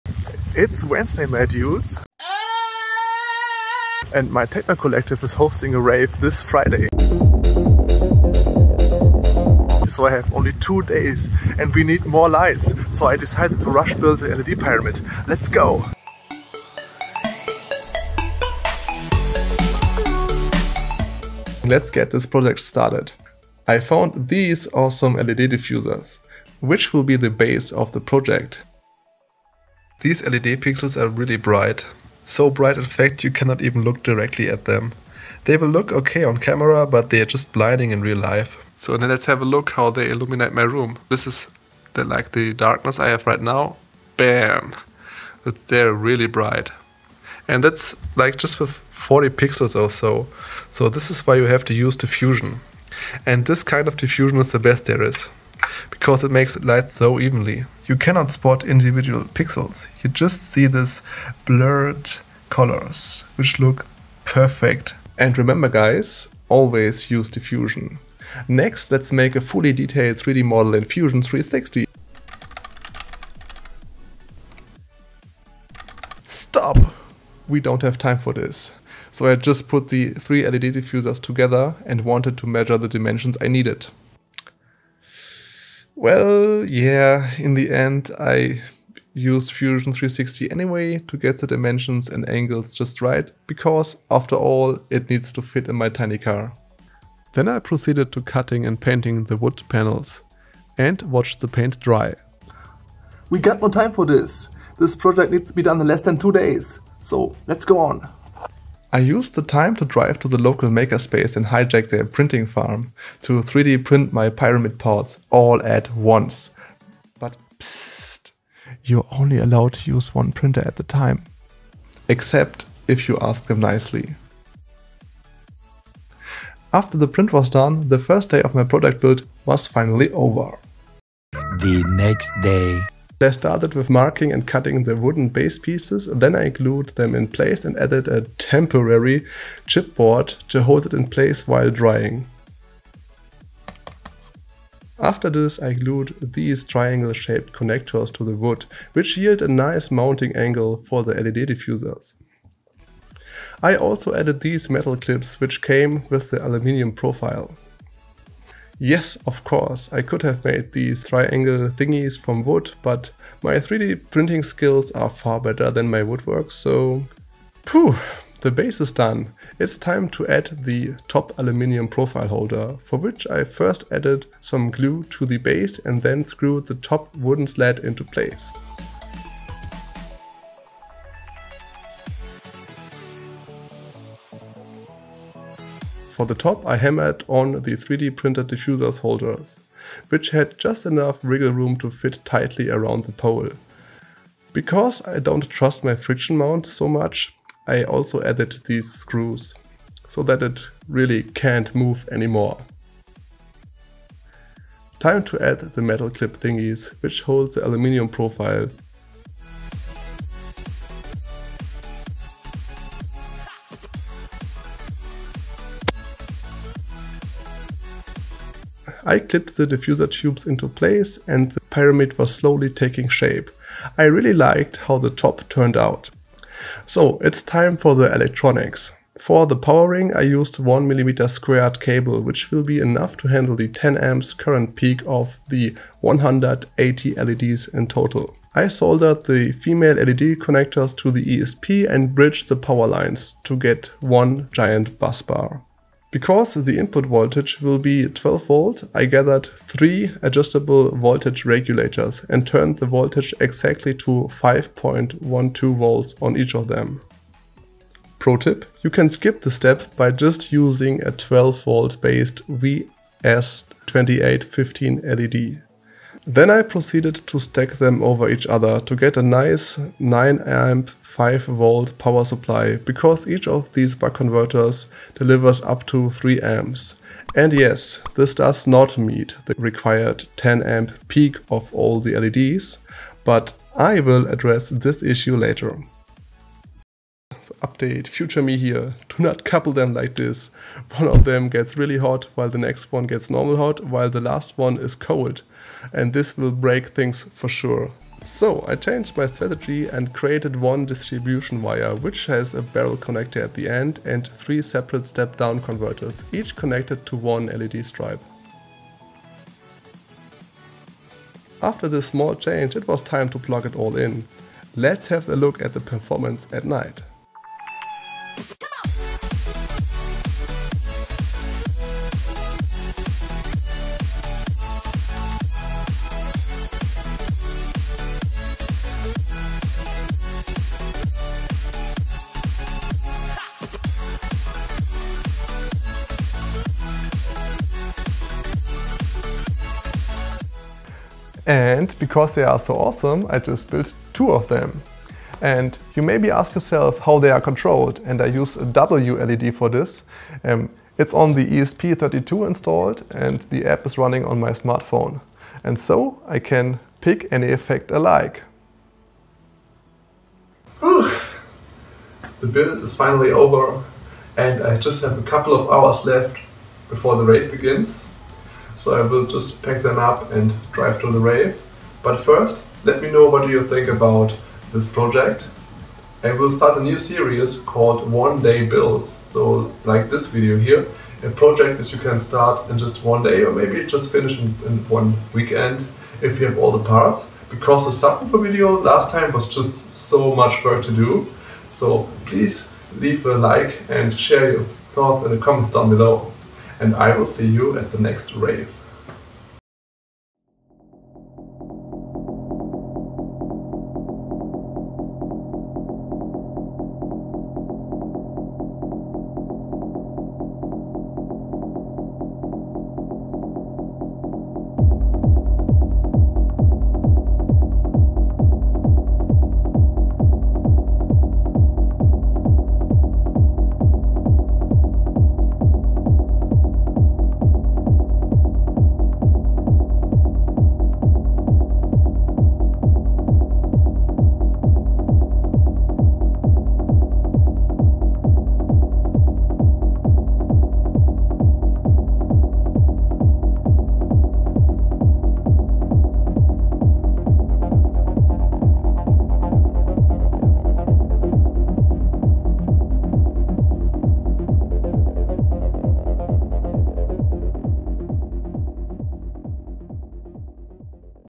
How I built Pyramids out of 180 LEDs ✨
I made some awesome DIY LED Pyramids! 180 individually addressable LEDs will light up your home or any rave. Best thing is, it only took 2 days to finish this project (and 2 months cutting😅).

My first video from my new series onedaybuilds hope you like it, feedback is always appreciated.

Special thanks to Maurice Jeliński providing the awesome footage from our rave ♥

🧰 Stuff i used:
- 3m WS2812B LEDs 60px/m
- Milky aluminium LED diffusors
- ESP32
- Wooden slats
- Chipbaord
- 1mm² wire
- 3x LM2596 step down module
- 12V 90W powerbrick

- Soldering Iron
- Wood saw
- Drill